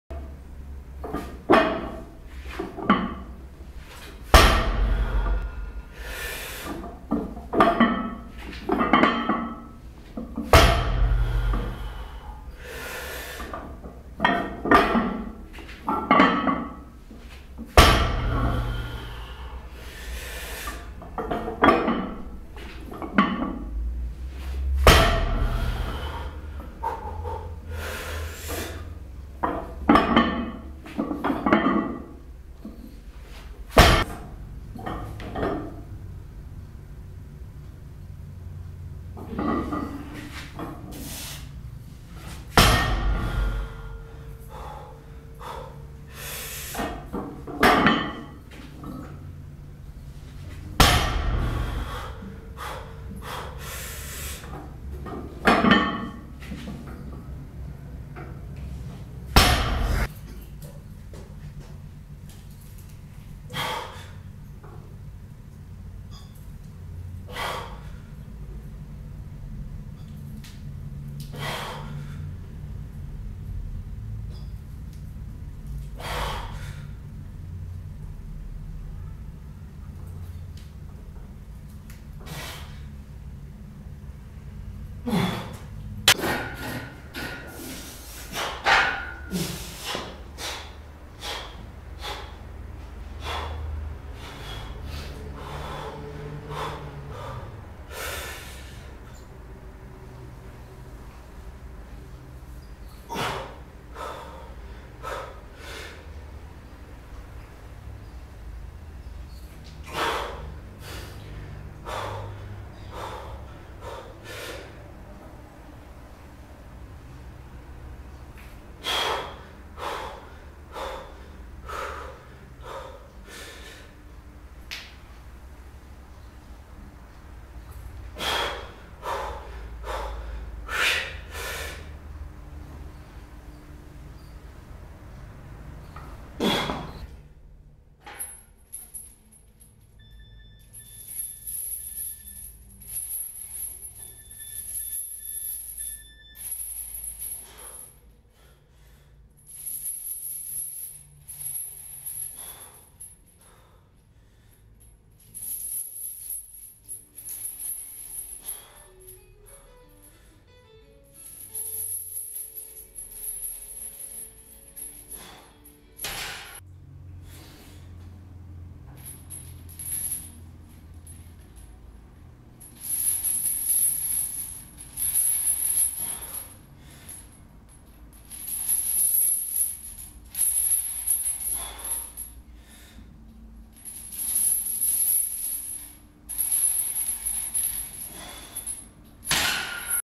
staccofrontpanca
stacco deficit 7cm 125kg
military 55
front 90
panca stretta 75+10catene